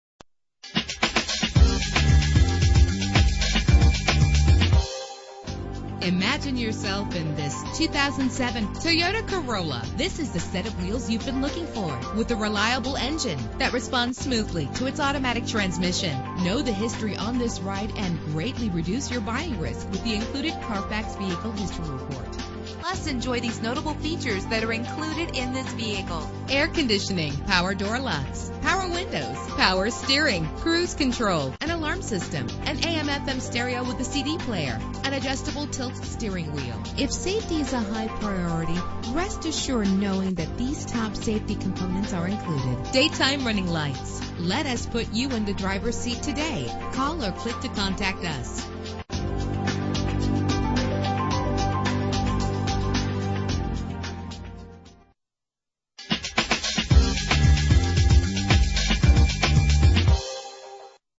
2007 Toyota Corolla - Middle Island NY
Year: 2007
Make: Toyota
Model: Corolla
Trim: S Sedan 4D
Engine: 4 cylinder 1.8 Liter
Transmission:
Color: Gray
Mileage: 54082
Address: